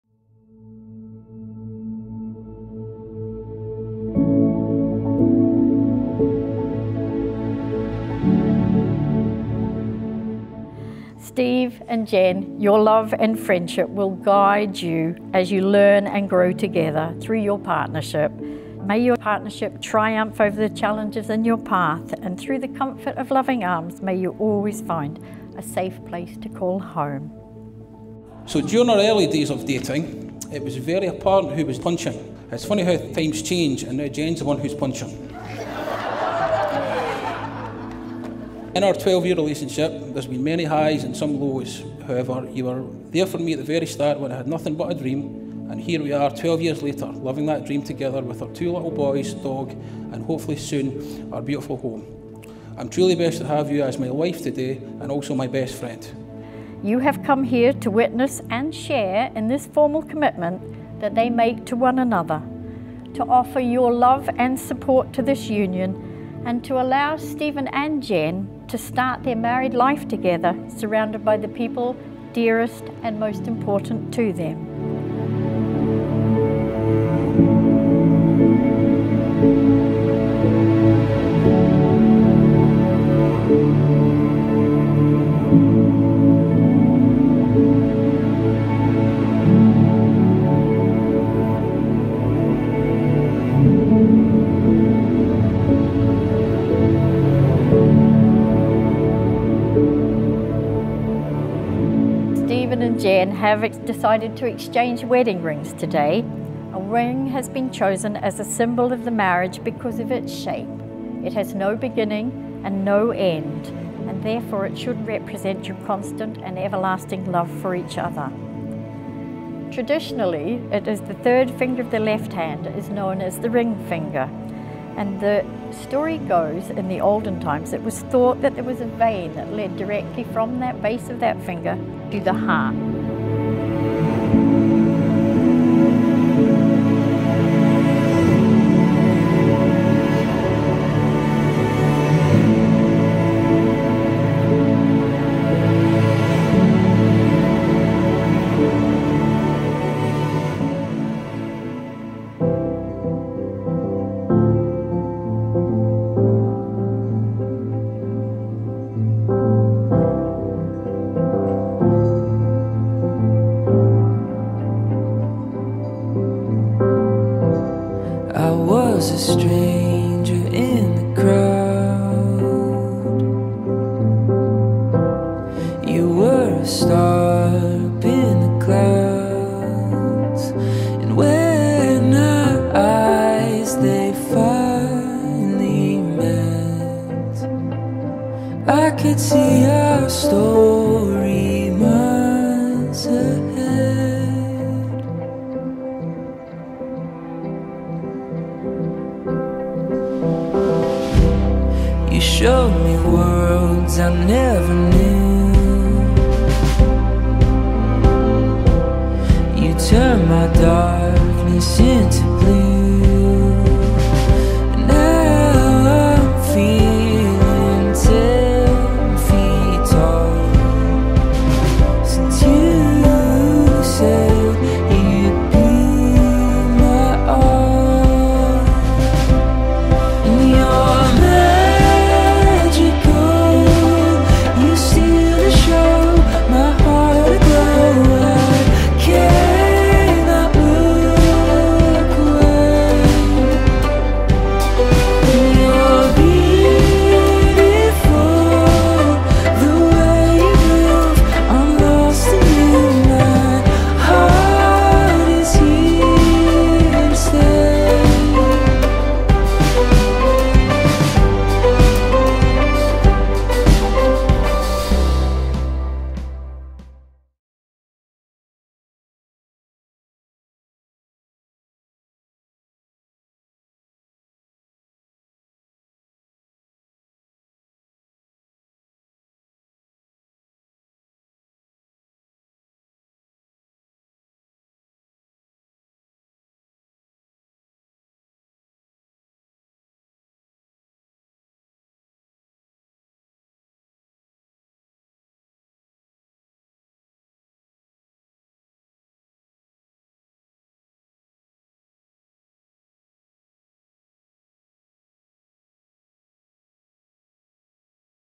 Jen & Steven's Wedding Day Film
Filmed at Atholl Palace 11th August 2018